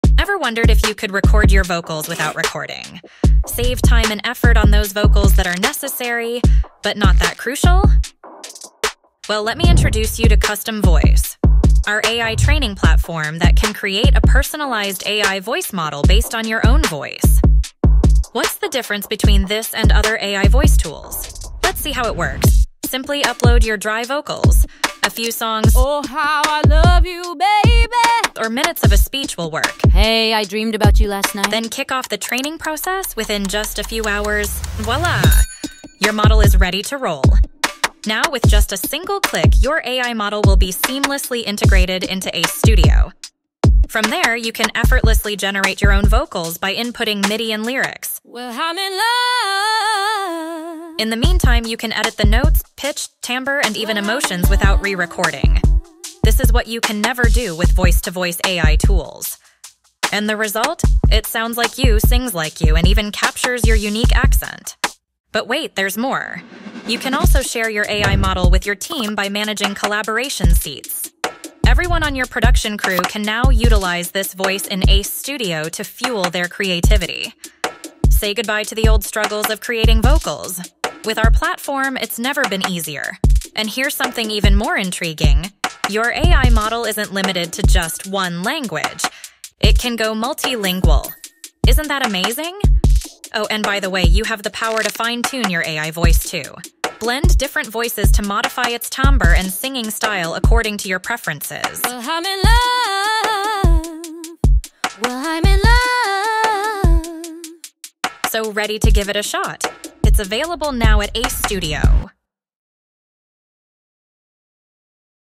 Customize Your Own Voice with ACE Studio
Ever wondered if you could record your vocals without recording? Save time and effort on those vocals that are necessary, but not that crucial?

Well, let me introduce you to 'Custom Voice'—our AI training platform that can create a personalized AI voice model based on your own voice.

00:00 Introduction
00:17 Difference from Other AI Voice Tools
01:04 Share Your AI Models with Collaboration Seats
01:23 Multilingual & Fine-tune